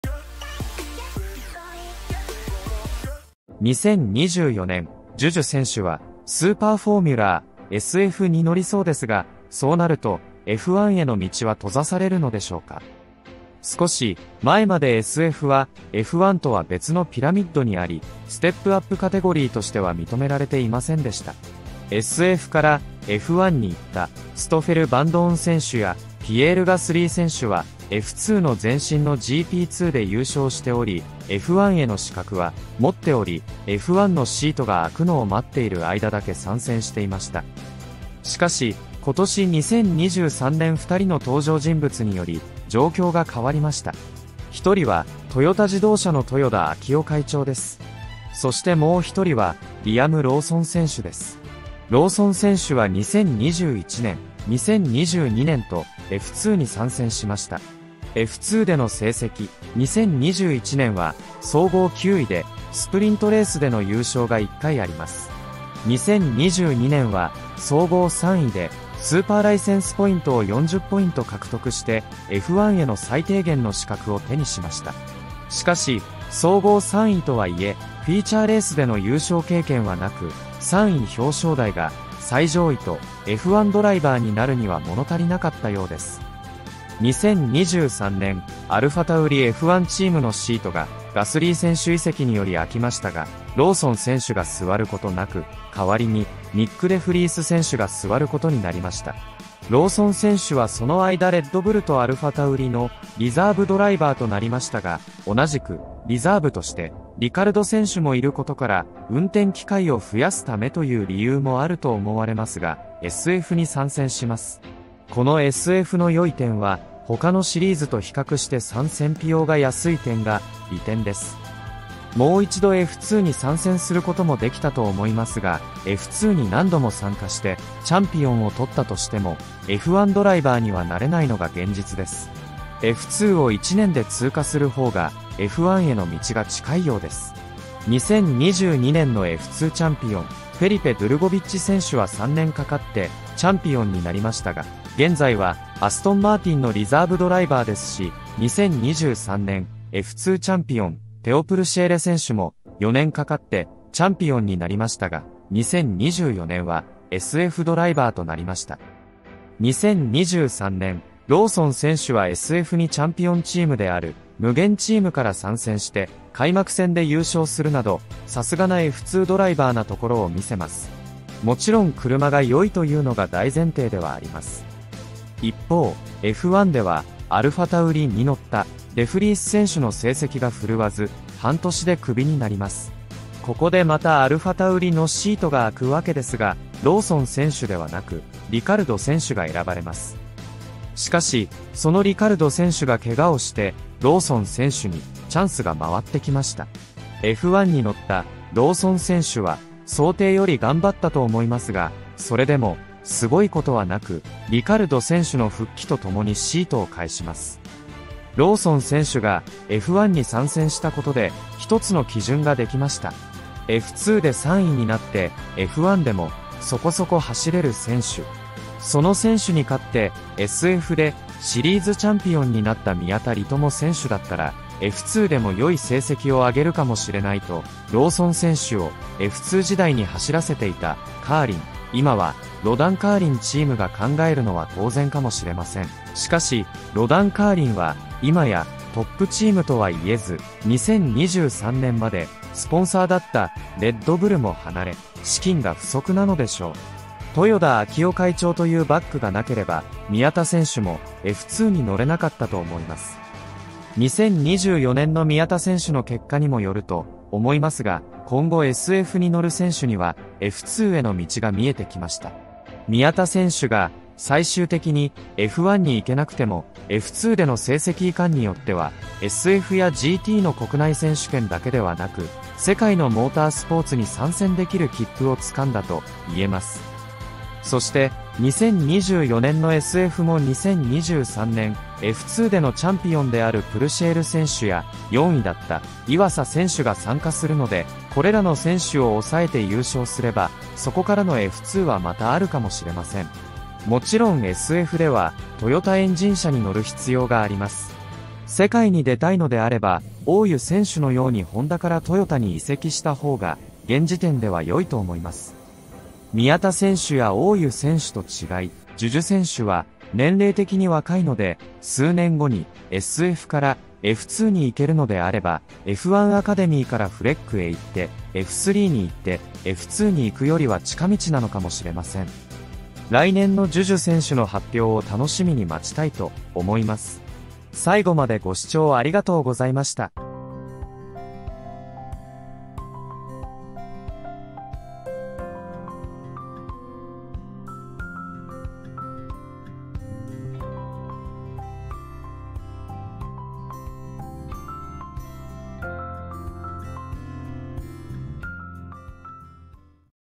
【Juju選手】F１は無いのか？【SuperFormula】
2024年Juju選手はスーパーフォーミュラー（SF）に乗りそうですが、
そうなるとF1への道は閉ざされるのでしょうか？

【Juju選手】ファンミーティング監督にあのモヤモヤを聞きました！【夏休み】

【Juju選手】SFからF1へ行く方法はあるのでしょうか？【お応えシリーズ】